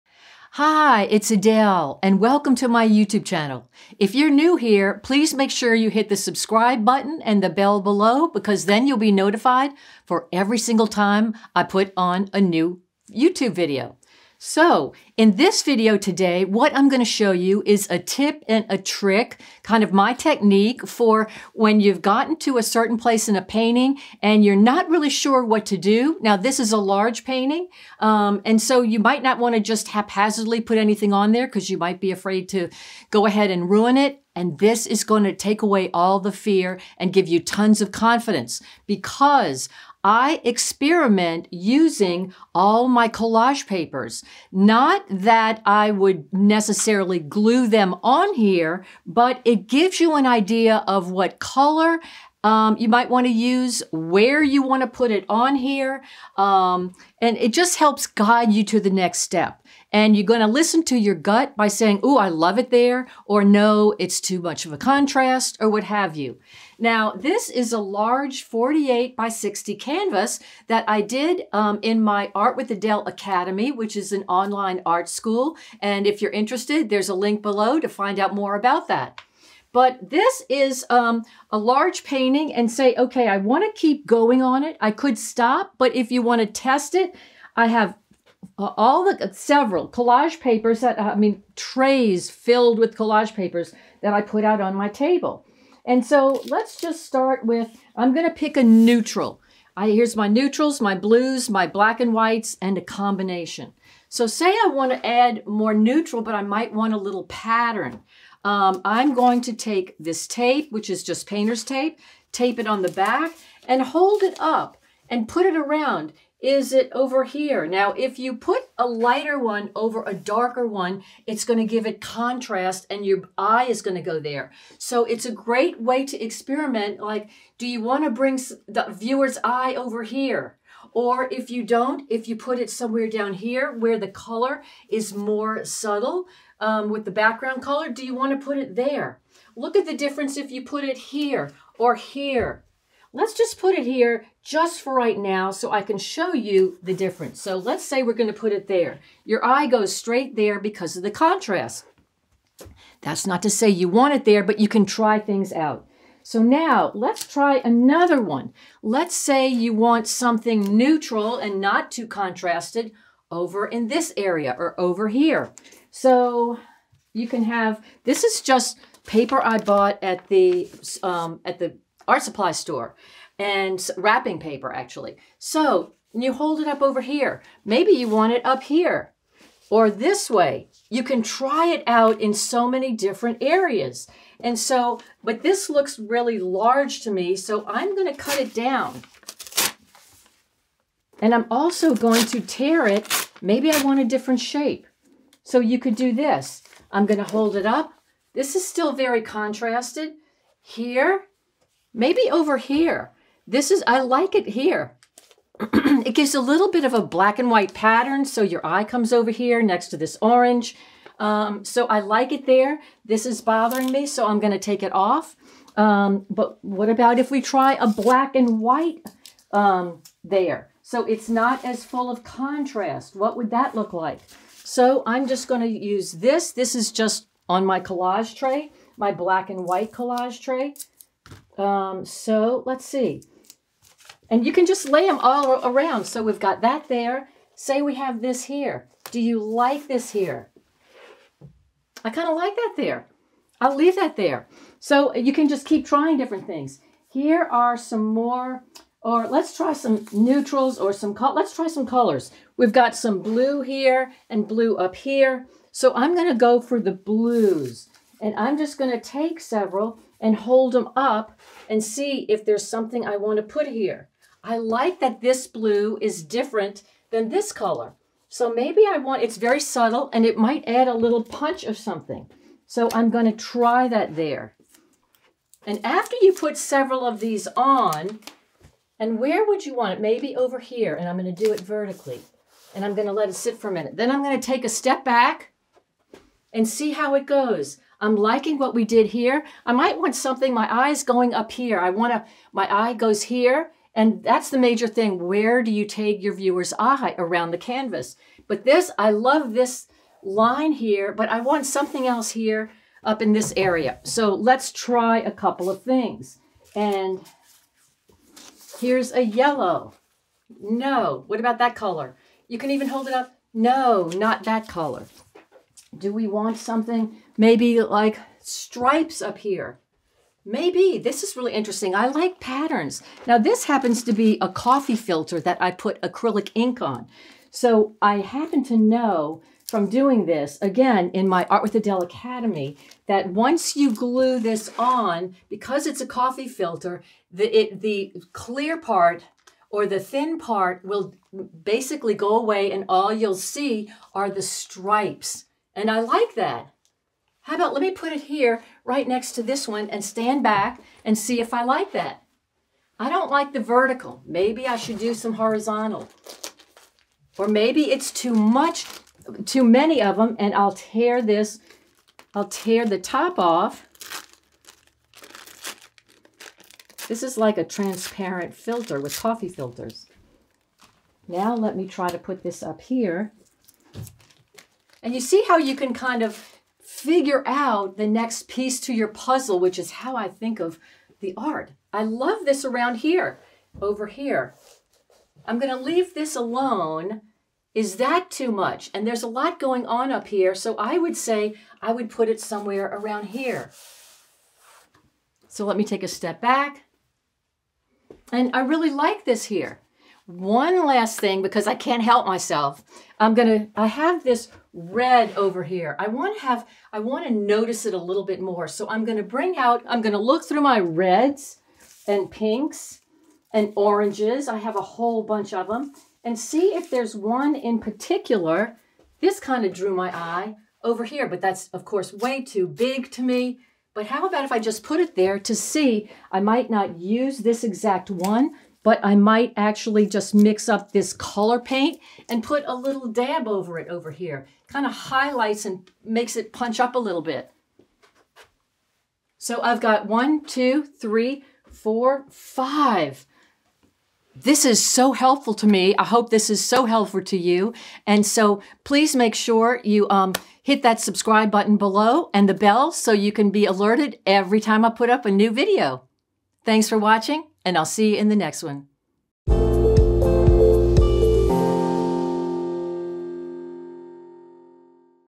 Using Collage as Painting Guides / Art with Adele
Using Collage as Painting Guides / When you're not sure where to take your abstract painting try using collage pieces as guides! In this video, I put out several of my restaurant trays filled with my collage papers and I scan them for either a shape that interests me, a pattern that seems like it might work, or a color that would be the perfect fit. Then I hold it up to the painting, move it all around and pay attention to where my intuition is leading me. This process removes the fear of making mistakes.